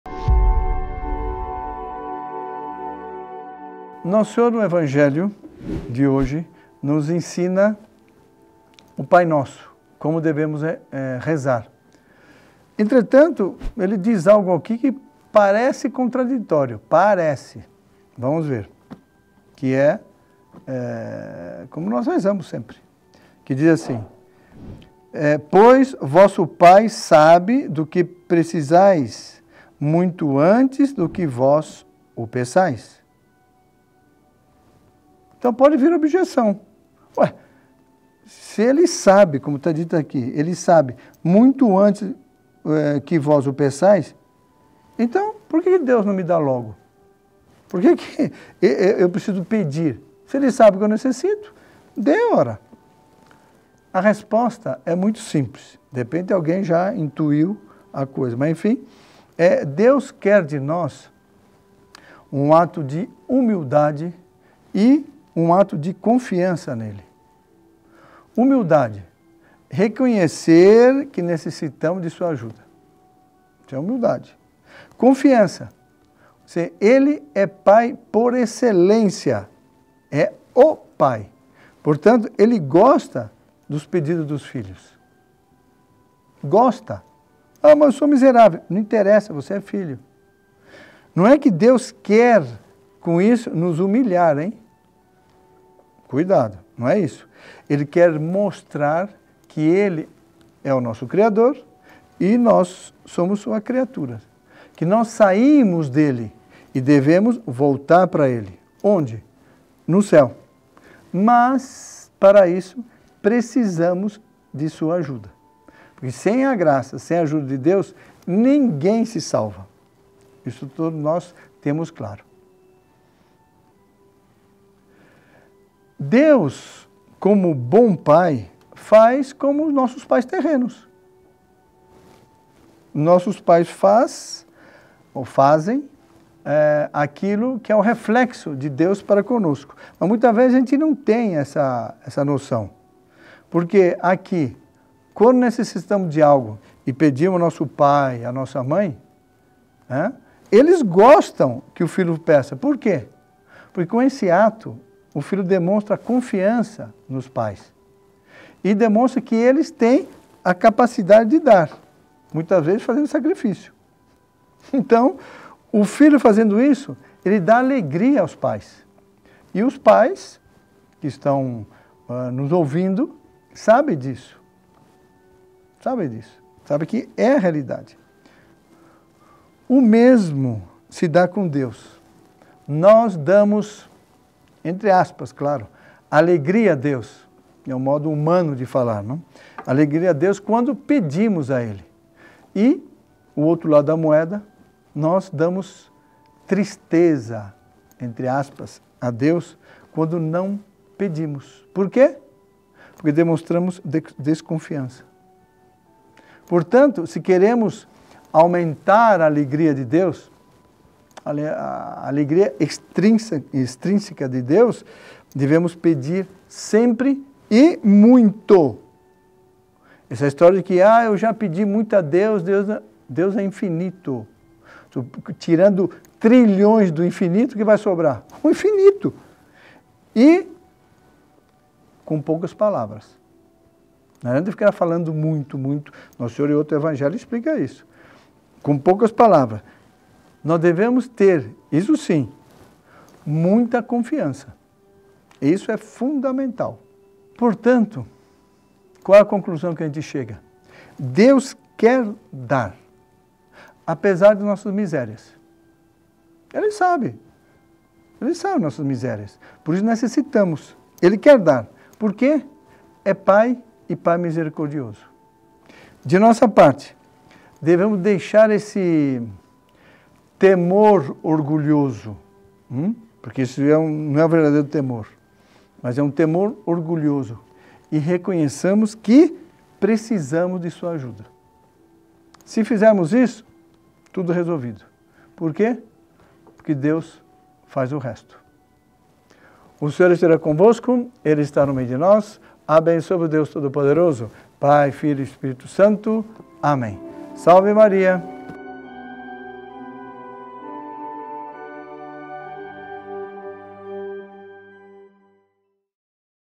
Homilia diária I Deus se alegra em dar! I Pe. Roberto Polimeni (20 fev. 2024)
Terça-feira I 1ª Semana da Quaresma

Naquele tempo, disse Jesus aos seus discípulos: “Quando orardes, não useis muitas palavras, como fazem os pagãos. Eles pensam que serão ouvidos por força das muitas palavras.

Não sejais como eles, pois vosso Pai sabe do que precisais, muito antes que vós o peçais. Vós deveis rezar assim: Pai nosso que estás nos céus, santificado seja o teu nome; venha o teu Reino; seja feita a tua vontade, assim na terra como nos céus. O pão nosso de cada dia dá-nos hoje. Perdoa as nossas ofensas, assim como nós perdoamos a quem nos tem ofendido, e não nos deixes cair em tentação, mas livra-nos do mal.

De fato, se vós perdoardes aos homens as faltas que eles cometeram, vosso Pai que está nos céus também vos perdoará. Mas, se vós não perdoardes aos homens, vosso Pai também não perdoará as faltas que vós cometestes”.